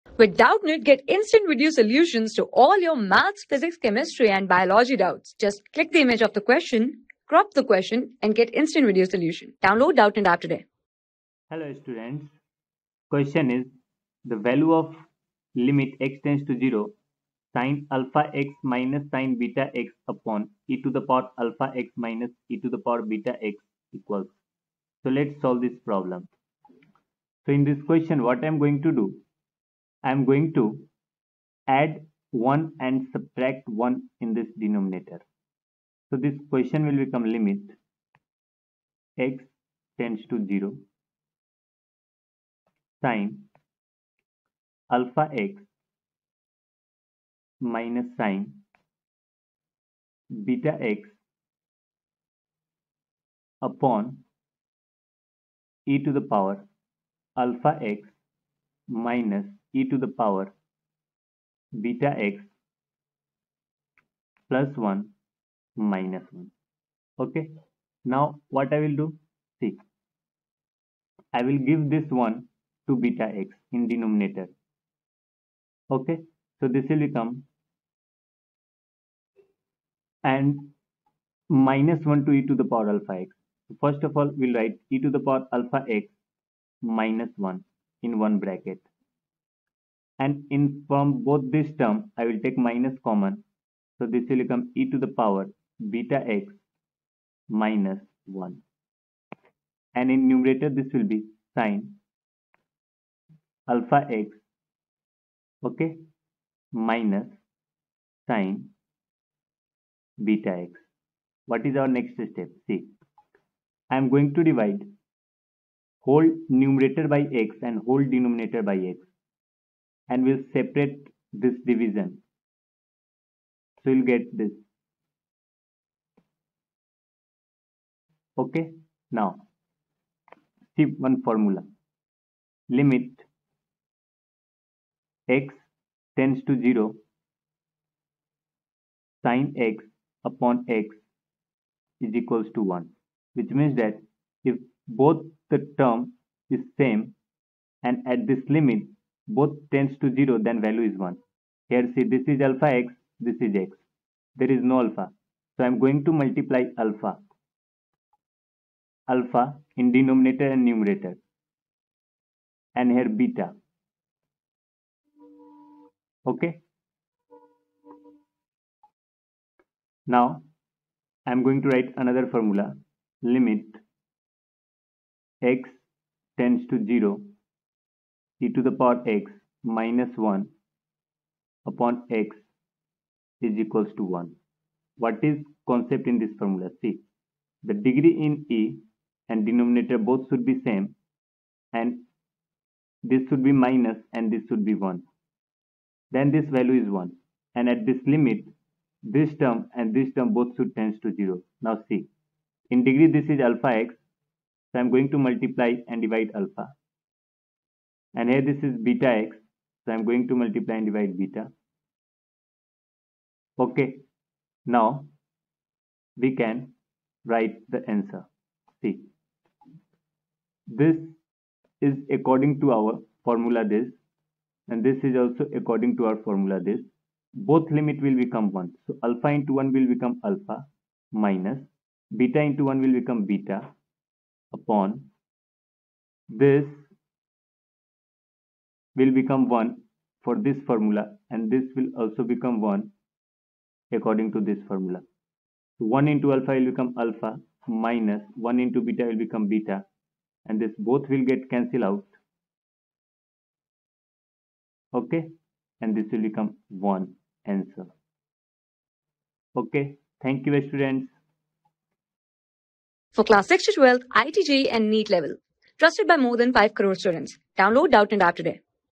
The value of lim_(x to 0)(sin alpha X - sin beta x)/(e^(alphax) - e^(beta x)) equals | 12 | LIMI...
The value of lim_(x to 0)(sin alpha X - sin beta x)/(e^(alphax) - e^(beta x)) equals
Class: 12
Subject: MATHS
Chapter: LIMITS AND DERIVATIVES
Board:IIT JEE

👉 Have Any Query? Ask Us.